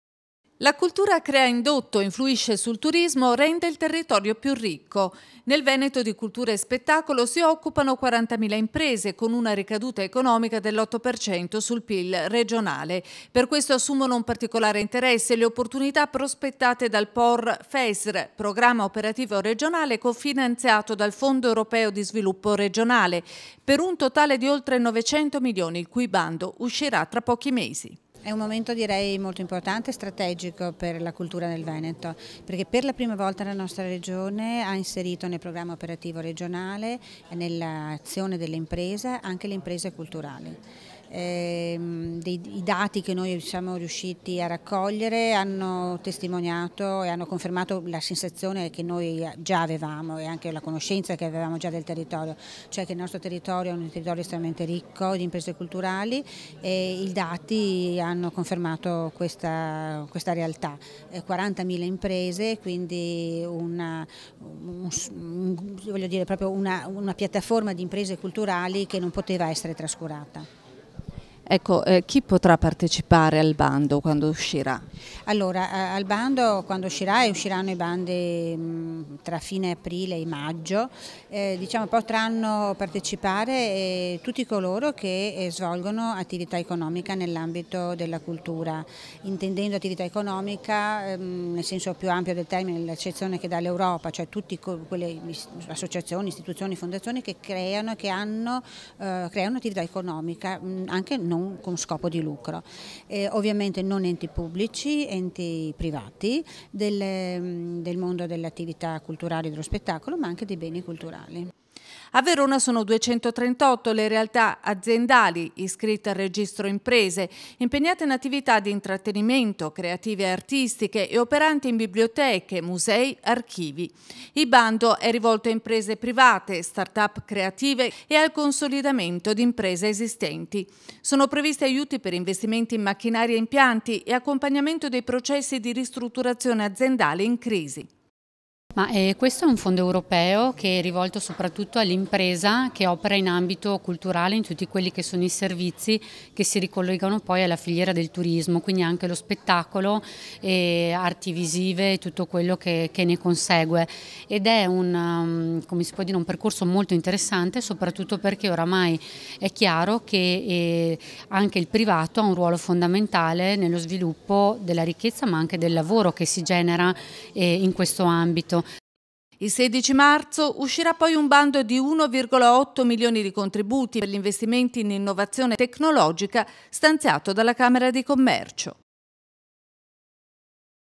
investire sulla cultura
La cultura crea indotto, influisce sul turismo, rende il territorio più ricco. Da questo presupposto nasce. Nel Veneto di cultura e spettacolo si occupano 40 mila imprese  con una ricaduta economica dell’8% sul PIL regionale. Per questo assume un particolare interesse le opportunità prospettate  dal POR Fesr , Programma Operativo Regionale (POR), cofinanziato dal Fondo Europeo di Sviluppo Regionale (FESR) per un totale di oltre 900 milioni, il cui bando uscirà tra pochi mesi.

MARIA TERESA DE GREGORIO
Direttore Dip. Cultura Regione Veneto

SILVIA NICOLIS
Camera di Commercio – Verona